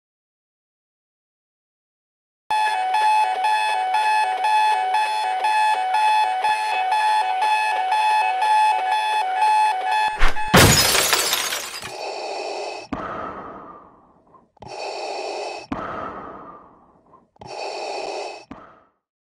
Star Wars SVJ Episode 1
Episode 1.

 Aujourd’hui, SVJ attend un invité très spécial…
Mais aura-t-il la Force de se lever pour son premier jour à la rédaction?

Le 16 décembre, Star Wars débarque aussi dans Science & Vie Junior !